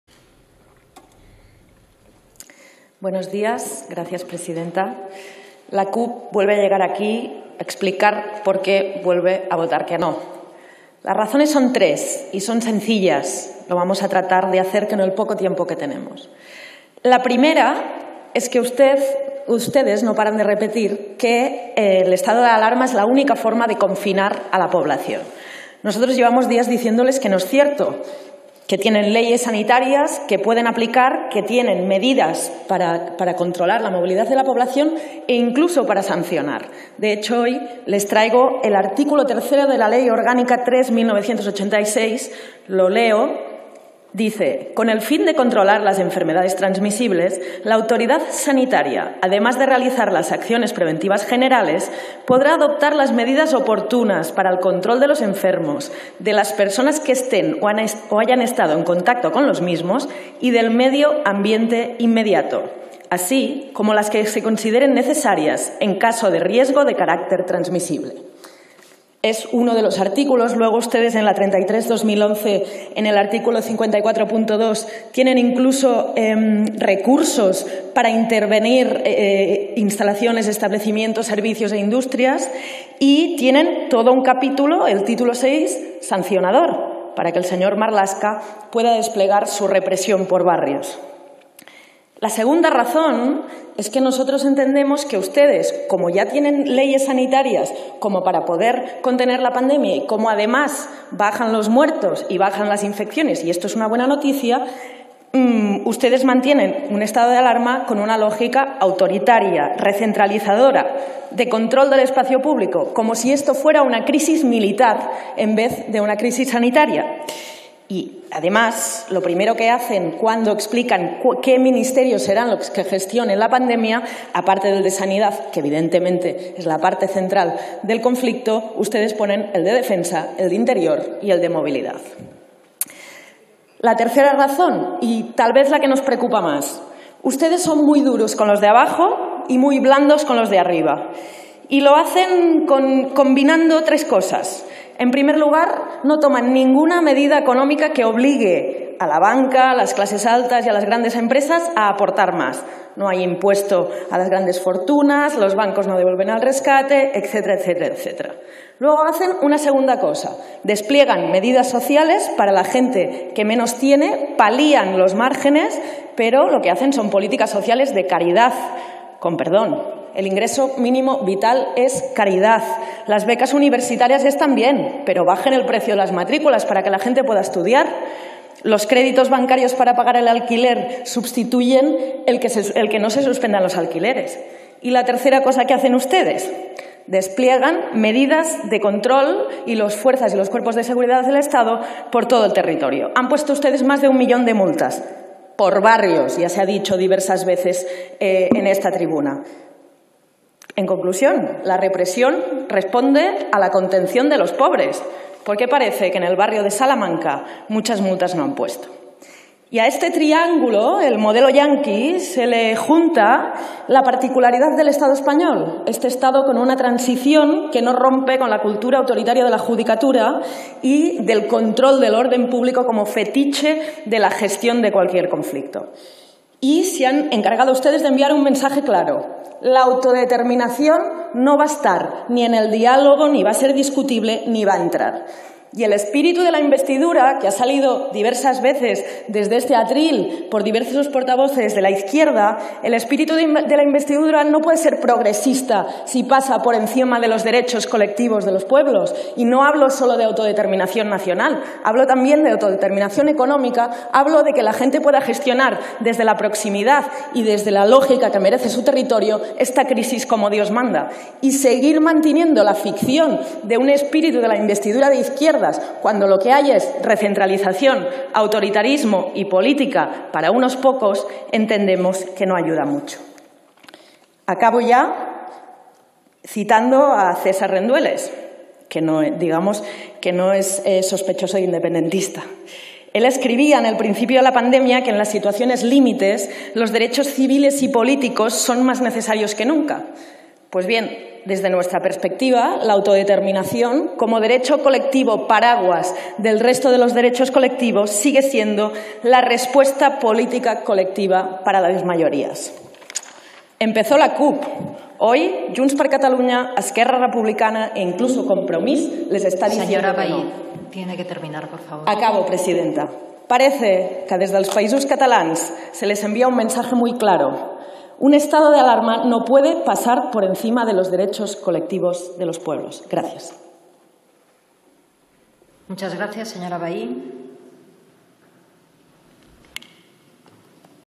Mireia Vehi (CUP) Prorroga estado de alarma
Solicitud de autorización de la prórroga del estado de alarma declarado mediante el Real Decreto 463/2020, de 14 de marzo, por el que se declara el estado de alarma para la gestión de la situación de crisis sanitaria ocasionada por el COVID-19, prorrogado por los Reales Decretos 476/2020, de 27 de marzo, 487/2020, de 10 de abril, 492/2020, de 24 de abril, y 514/2020, de 8 de mayo.